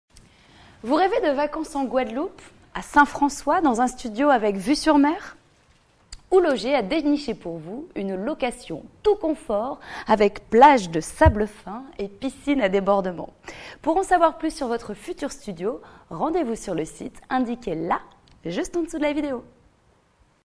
Guadeloupe St Francois Location Studio Vue mer
Vous rêvez de vacances en Guadeloupe, à Saint François, dans un studio avec vue sur mer ?
OULOGER a déniché pour vous une location tout confort avec plage de sable fin et piscine à débordement…
Dans un domaine de 7 hectares avec gardiennage, nous vous proposons notre studio avec vue sur mer au 1° étage, pour 2 ou 3 personnes : chambre climatisée, Wi-Fi à l'intérieur de l'appartement (chèques vacances acceptés).
Le Domaine de l'Anse des rochers est à 3 km du centre-ville et du port de pêche de Saint François, en Guadeloupe.
Saint François : la station balnéaire de Guadeloupe, un lieu paradisiaque… Venez découvrir son golf, sa marina, ses marchés aux épices, son casino, son aéro-club, ses activités nautiques, ses plages ensoleillées…
Pour en savoir plus sur votre future location, l'équipement de votre studio avec vue sur mer, les tarifs et disponibilités, rendez-vous sur le site…

"Aidez Sofian dans son combat contre l'ataxie telangiectasie"